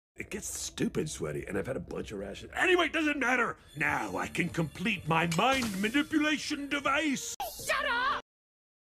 Mother's Day RV: Sheryl Goodspeed Tells Slade To Shut Up
My 3rd Slade Video